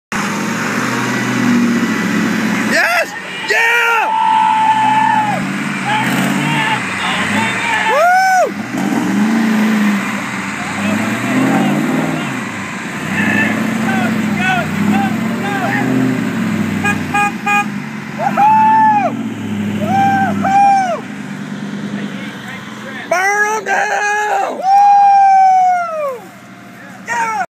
Bogged down Chevy
Finally getting my buddys bogged down truck out of the mud after about 12 hours